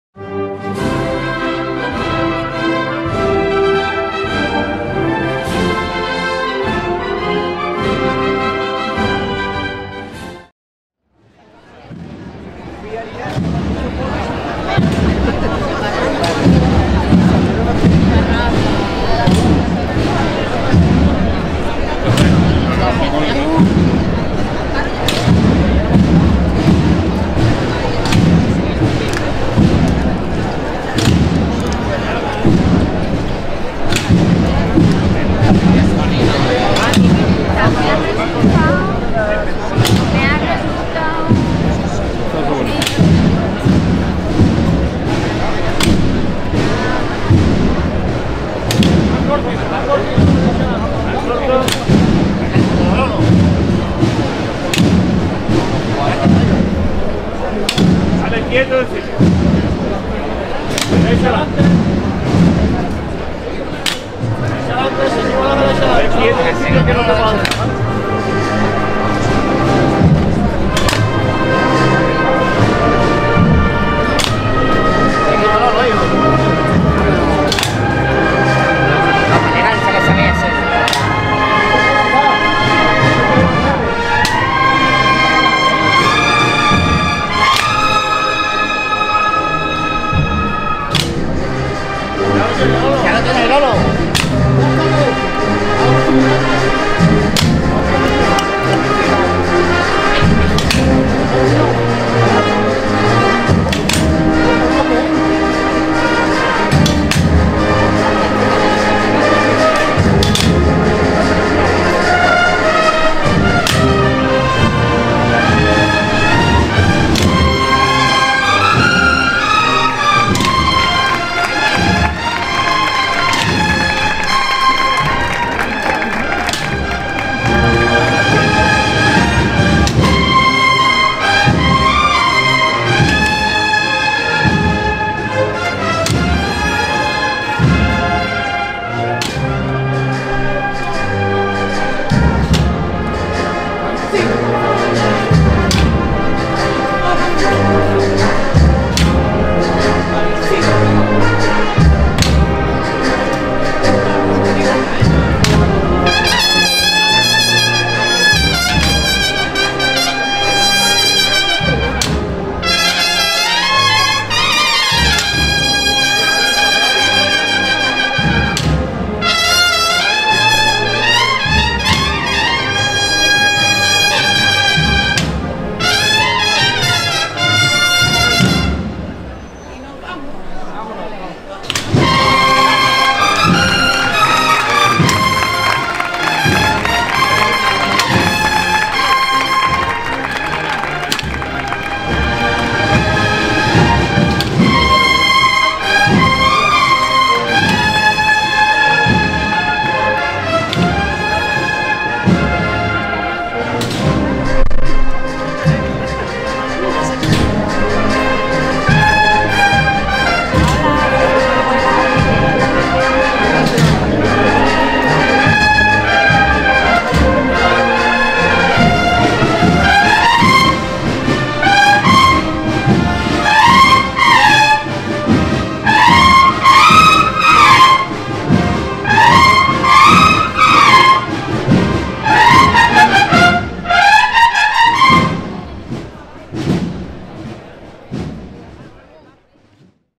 (4K) SENTENCIA EN LA PLAZA DE LA CATEDRAL |MAGNA CADIZ 2022
BANDA
Banda de Cornetas y Tambores del Nazareno de Huelva
FECHA
17 de septiembre de 2022